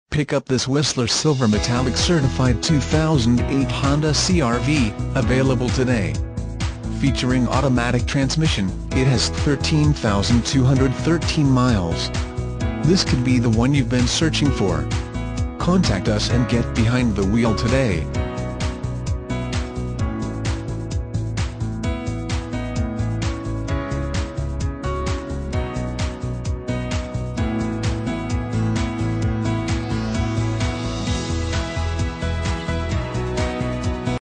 2008 HONDA CR-V
2008 HONDA CR-V Littleton CO

Pick up this Whistler Silver Metallic Certified 2008 Honda CR-V, available today. Featuring automatic transmission, it has 13213 miles. This could be the one you've been searching for.